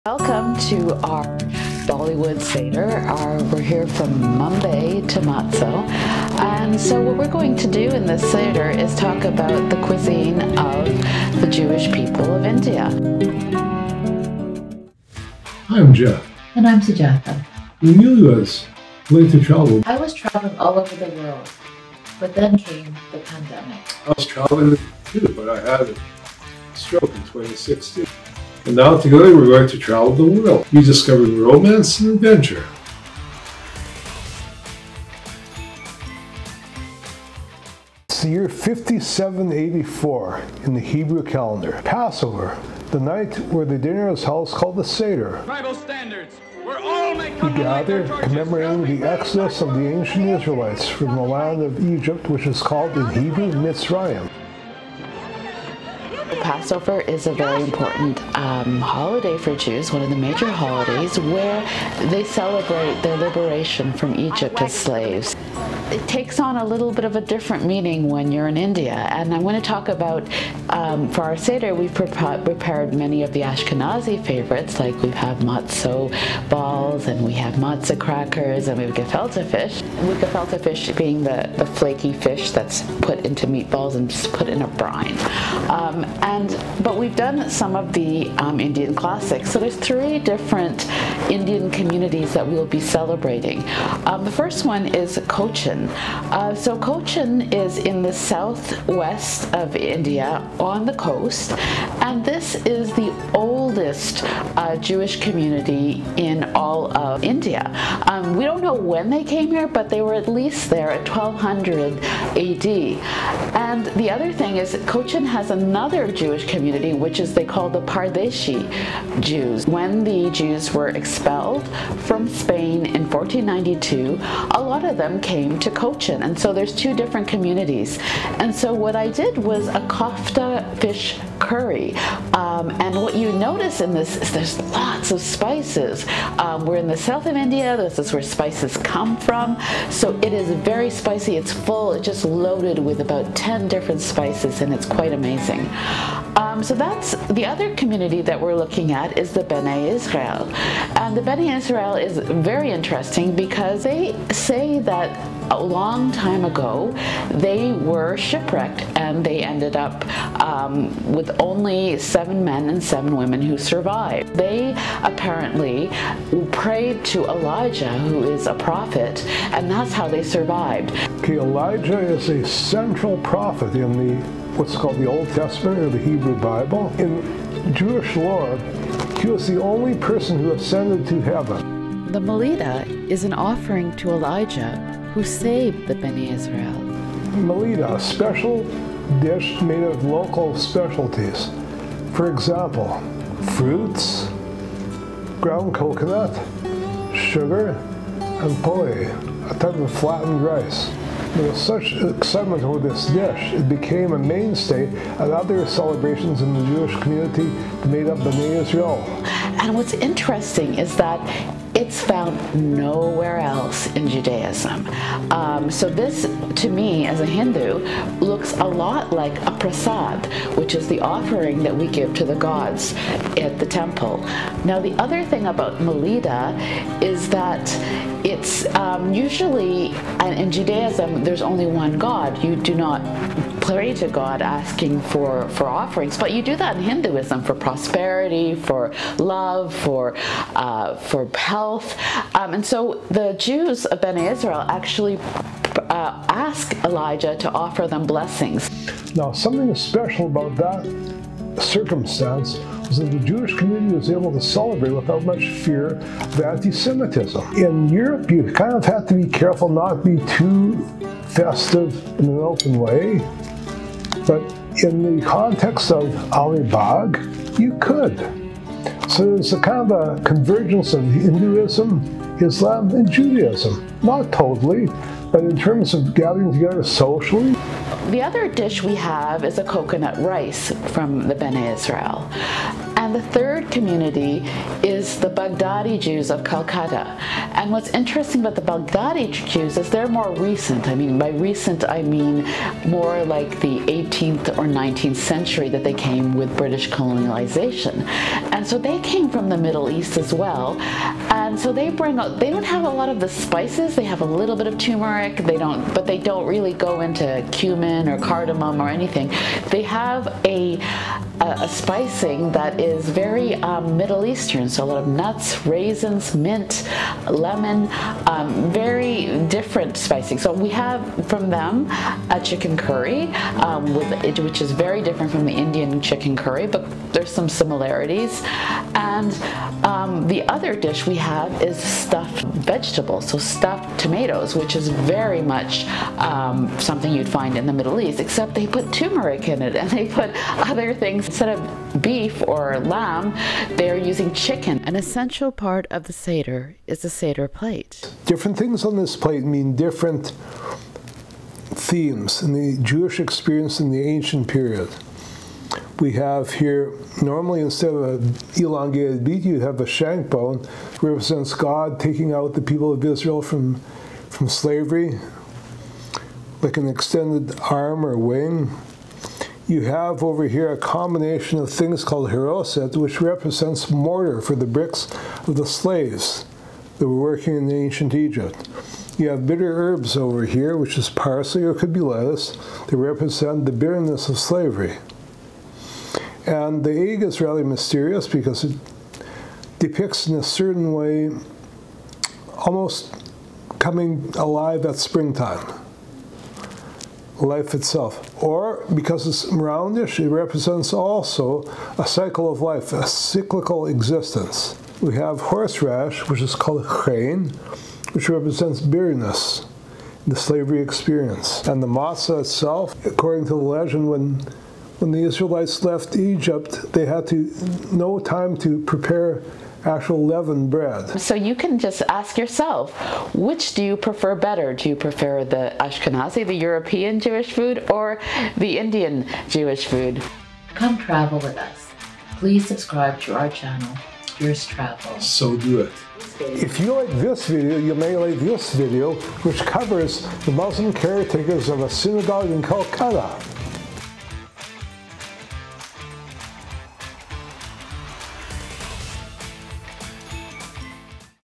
From Mumbai to Matzo: Our Bollywood Passover
Passover is one of the most important festivals in Judaism.  Since Jeff is Jewish, and Sujata is Hindu, they had to do a Passover that represents their traditions

From the bustling streets of Mumbai to the tranquil shores of Cochin, they discover the ancient roots of the Cochin Jewish community, the miraculous shipwreck story of the Mumbai Bene Israel, and the culinary delights of the Baghdadi Jews of Calcutta. Find out a seemingly Hindu inspired ritual, brimming with flowers, nuts, and fruits that is a bedrock of the Bene Israel's belief and faith of the prophet, Elijah.

Through captivating narration and stunning visuals, they showcase the fusion of Ashkenazi favourites with Indian classics, offering a tantalizing glimpse into the mouthwatering dishes prepared for this special occasion. From kofta fish curry to coconut rice, each dish tells a story of resilience, survival, and cultural exchange.

Join them on a flavourful journey through the Indian Jewish traditions. In this episode, they delve into the cultural fusion of Bollywood and Passover, exploring how the rich tapestry of Indian-Jewish heritage manifests in their unique celebration of this significant holiday.

Uncover the stories behind the Seder plate and the symbolic significance of each element. D

Whether you're a food enthusiast, history buff, or simply curious about the intersection of Jewish and Indian cultures, this episode promises to delight and inspire.

5) Elijah: By Andrea di Bonaiuto - Web Gallery of Art: Image Info about artwork, Public Domain,